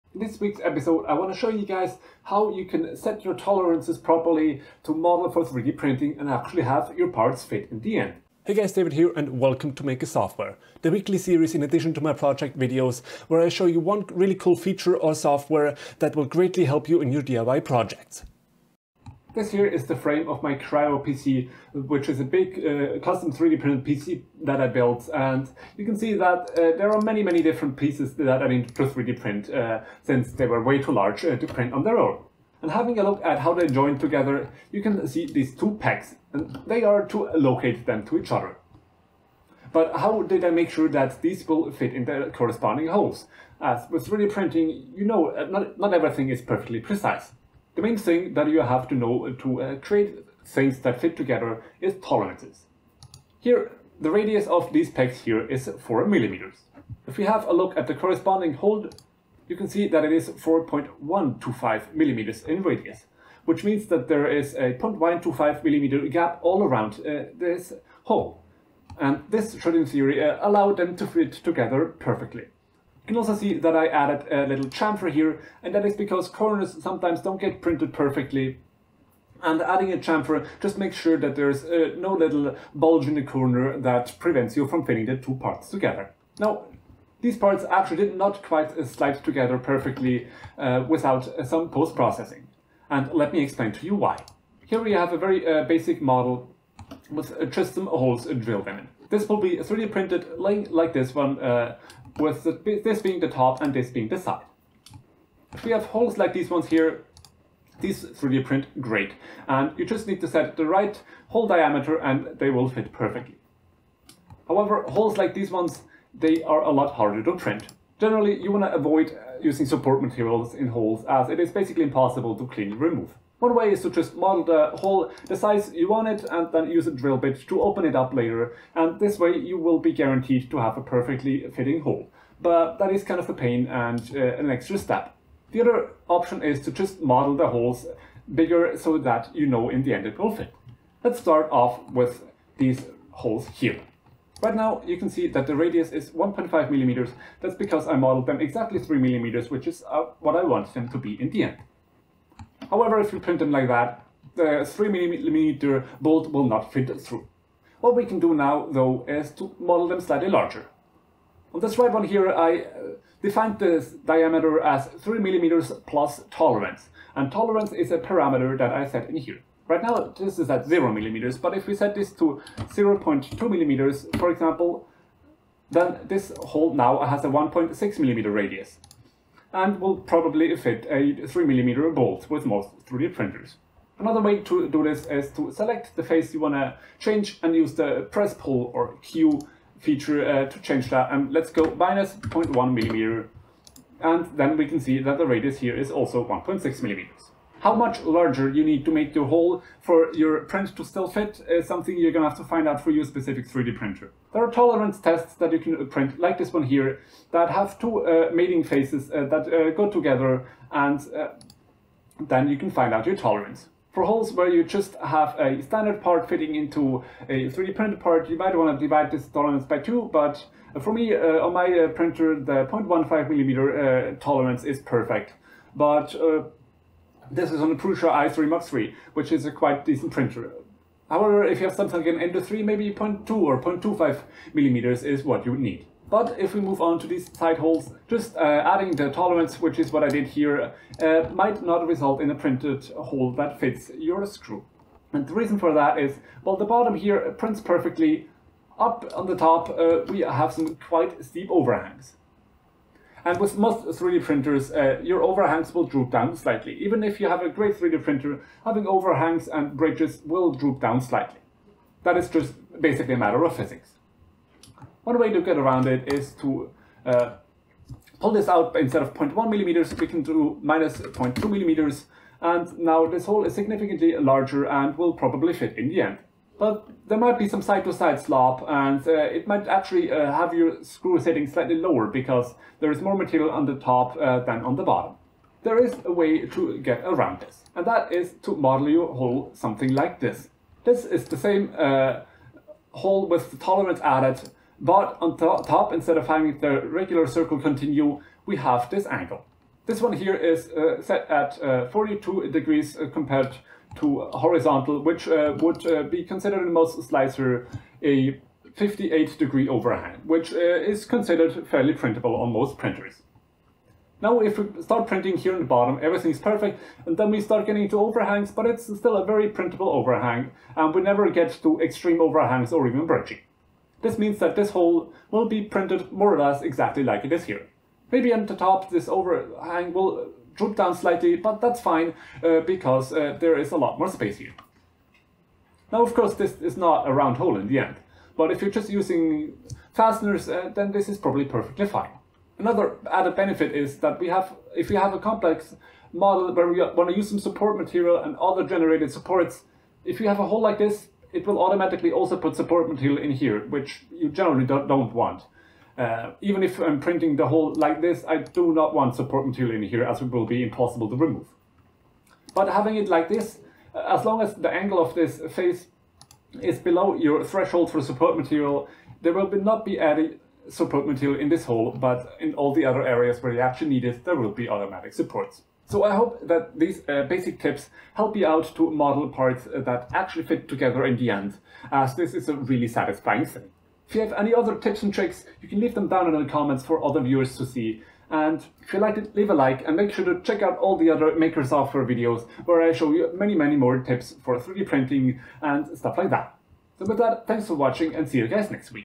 How to 3D PRINT Parts that FIT Together - Maker Software
Modeling things for 3d printing can be very satisfying. If you don't put the proper clearance, however, things will not fit together in the end, as 3d printing is not a perfect process. In this video, I show you some tips and tricks on how you can make your 3d prints actually fit together in the real world.

If you're looking for some good general-purpose advice on tolerances for 3d printed mechanical parts, here are the design rules that Voron design uses with great success:
-no overhangs over 45 degrees
-0.4mm chamfer on surfaces that touch the print bed (and no fillets on the build surface--breaks the 45 degree overhang rule)
-loose joints are generally oversized by 0.3-0.4mm diameter for a loose fit or 0.1mm for a press fit. 3.4mm for M3 hardware, 5.4mm for M5 hardware
-Countersunk holes "overhanging" the print surface should have a chamfer on them or another method like that to prevent issues
-Think about minimum perimeters/shells--try to avoid really thin features

Want to support me directly and look extra stylish?

Amazon (US)
Amazon (DE)